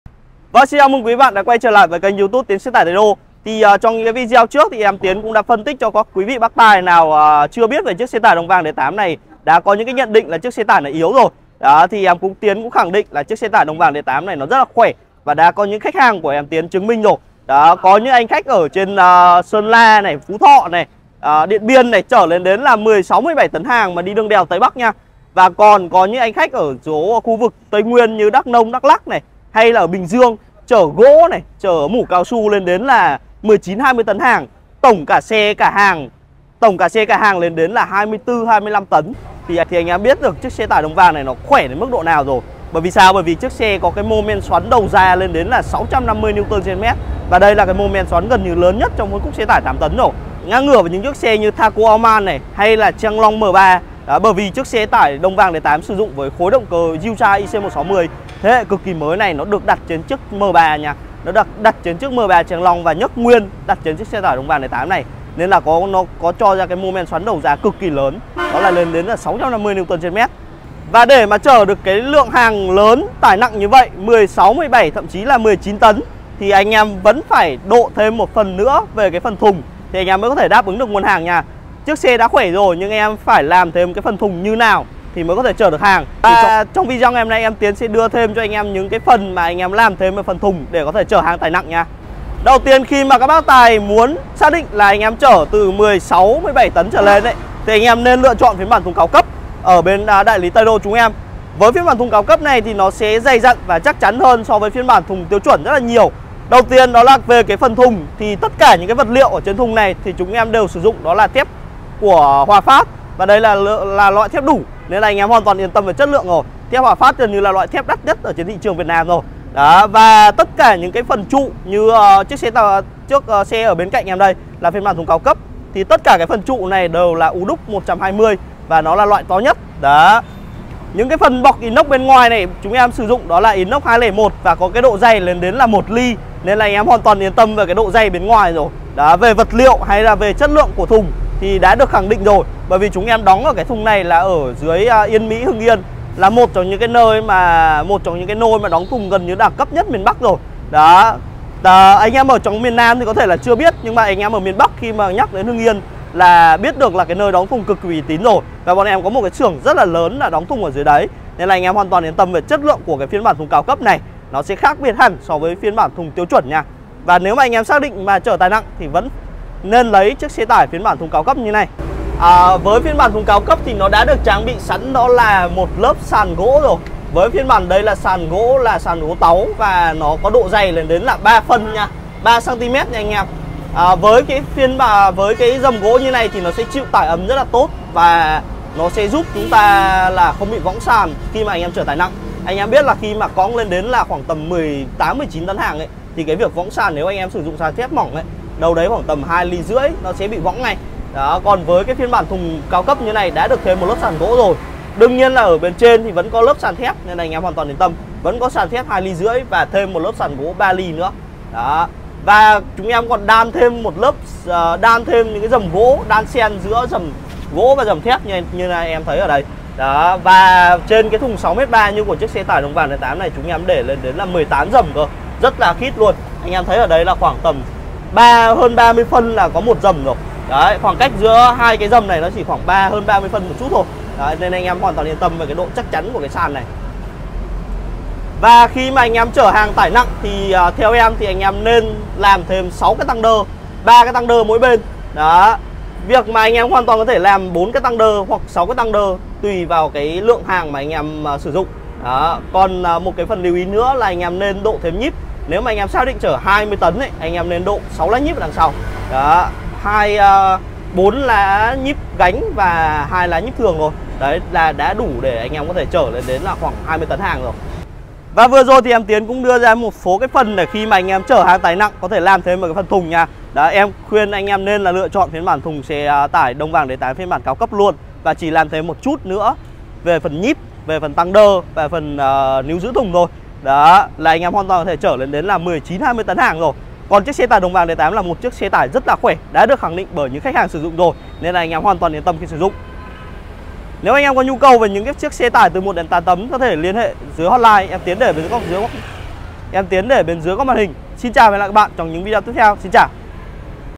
Xe tải Đồng vàng D8 chở 20 tấn cần làm những gì ???
TỔNG QUAN XE TẢI 8 TẤN THÚNG DÀI 6.3m ĐỒNG VÀNG D8 Đây là dòng xe 8 tấn với cabin kép, thùng dài 6.3m mới ra mắt của nhà máy ô tô Đồng Vàng, cấu hình hiện đại. Xe tải Đồng Vàng D8 8 tấn thùng dài 6.3M, sử dụng động cơ Yuchai 3.8cc công suất 160 mã lực. Moment xoắn cực đại 650 Nm ở dải vòng tua 1800 vòng/phút, nên mức tiêu hao nhiên liệu cực ký ấn tượng 10lit/100km. Mời Quý khách cùng trải nghiệm dòng xe tải Đồng Vàng D8 8 tấn thùng dài 6.3m. đây là dòng xe có tải trọng lớn nhất trong phân khúc xe tải thùng 6.3m.
Địa chỉ Showroom: 883 Quang trung, Ba la, Hà Đông, Hà Nội